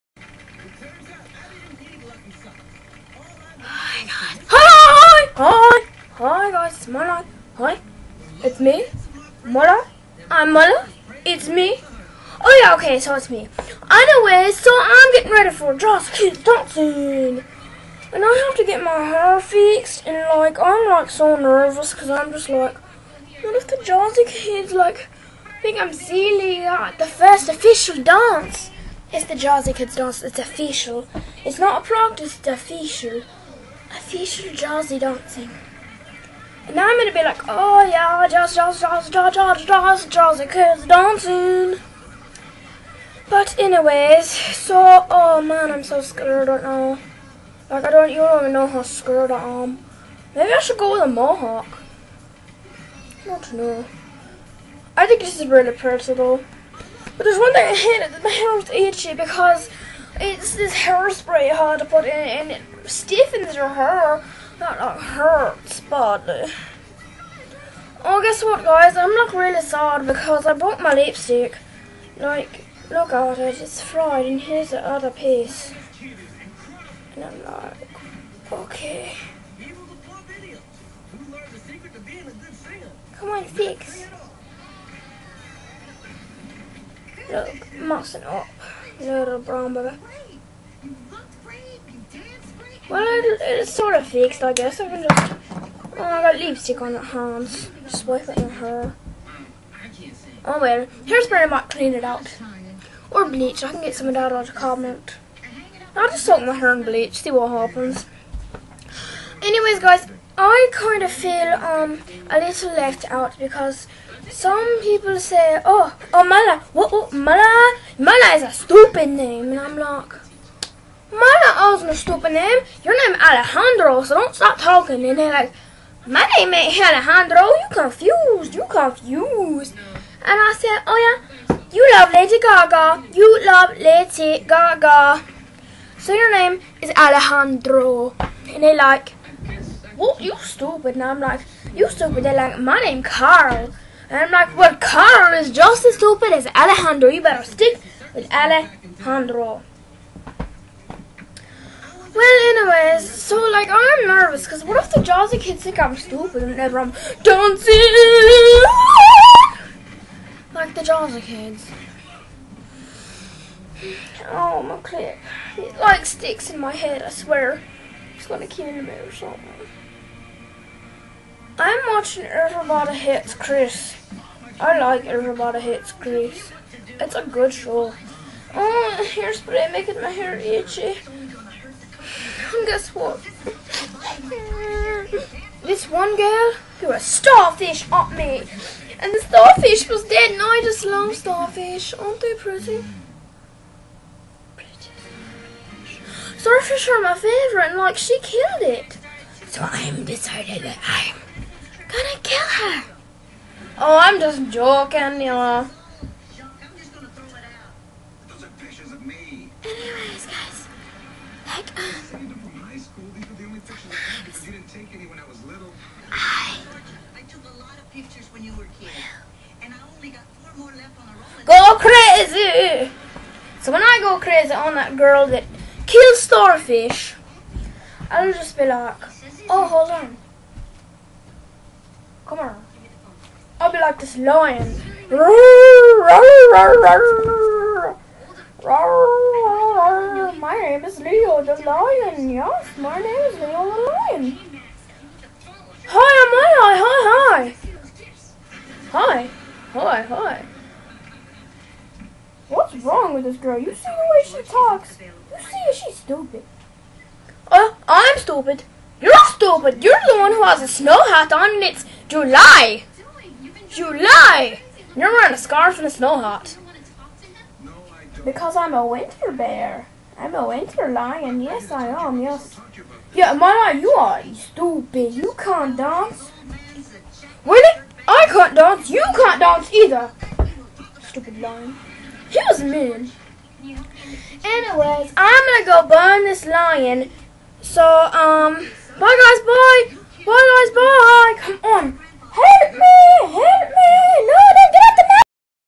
Mylie Vlog On: Hair Spray, Alejandro, Starfish, and The Lion
Mylie before her Jazzi Kids Dancing shoot talking about a little bit of everything.
MYLIE LINKS
ADD MYLIE ON FACEBOOK